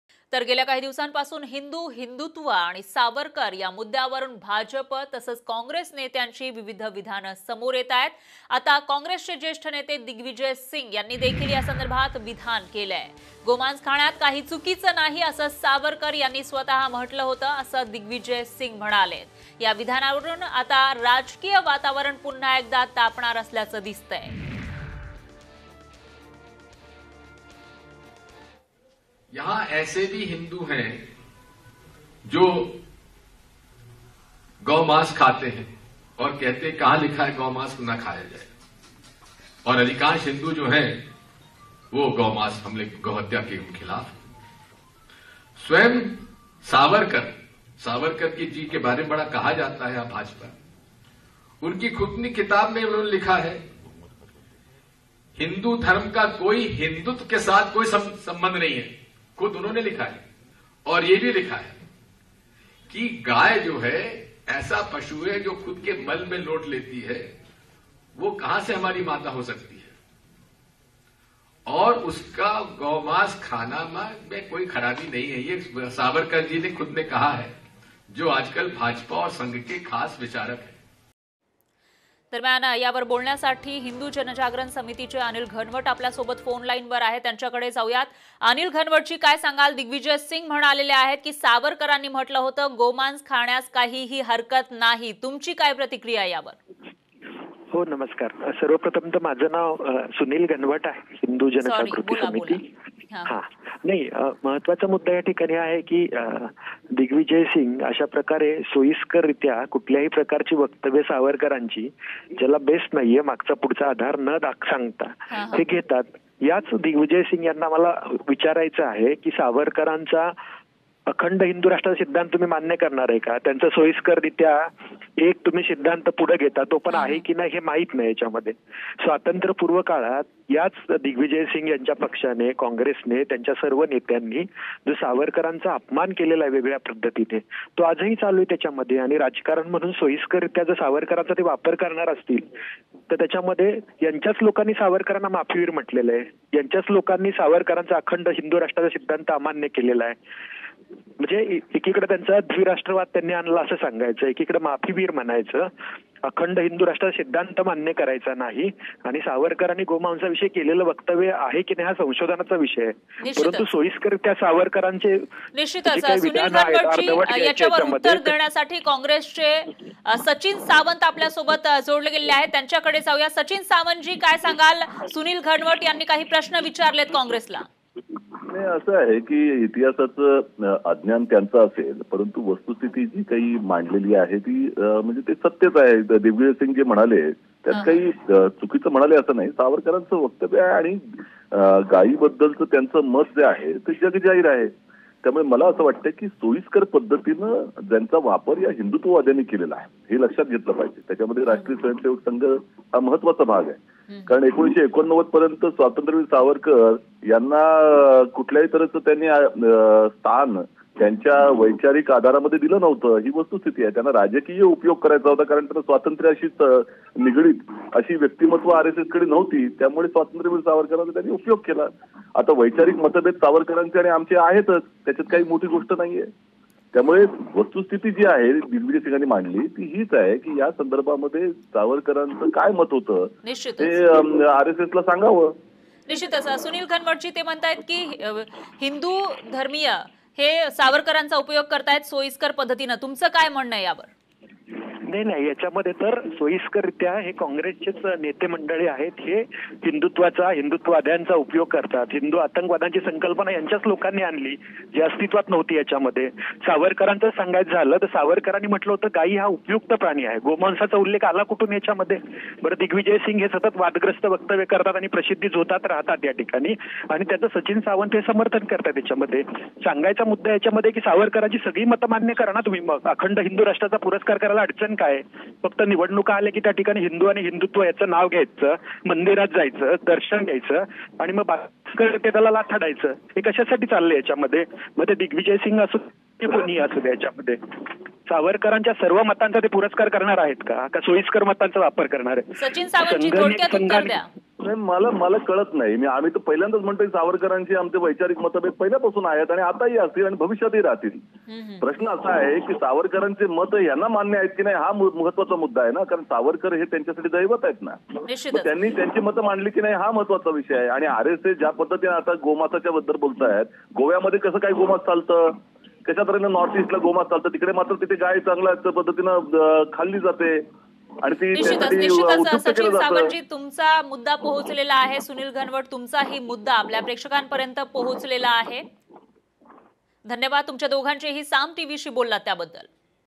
Digvijay Singh: "गोमांस खाण्यात गैर नाही' सावरकरांनी लिहिलंय" | Sakal Media
'गोमांस खाण्यात गैर नाही' सावरकरांनी लिहिलंय !;दिग्विजय सिहांच्या वक्तव्यामुळे वाद